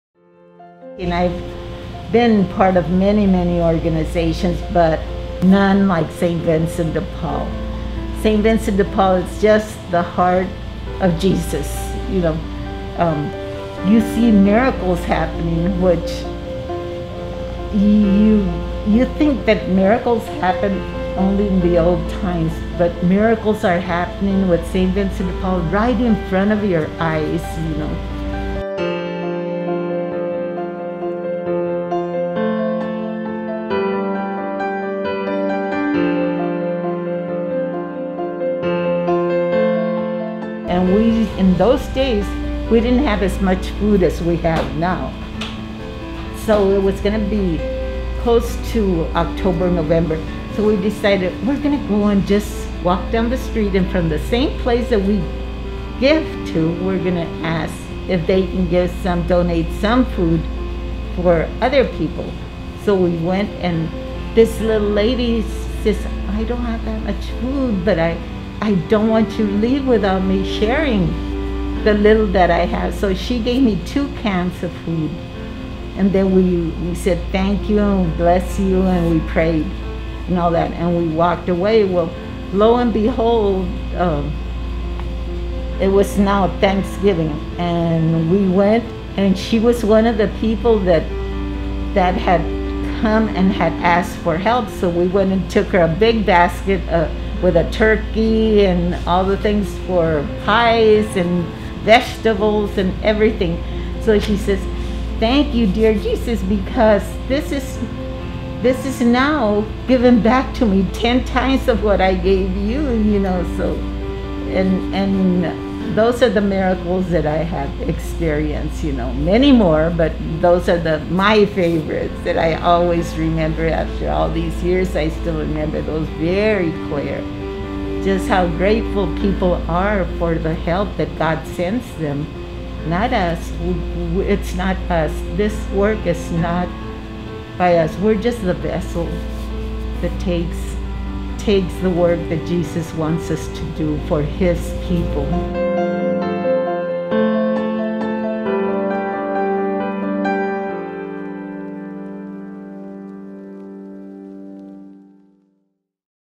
Celebrating miracles this Thanksgiving at St. Vincent de Paul
Serving the poor and giving others the opportunity to serve. St. Vincent de Paul has been helping families living in poverty in central and northern Arizona since 1946.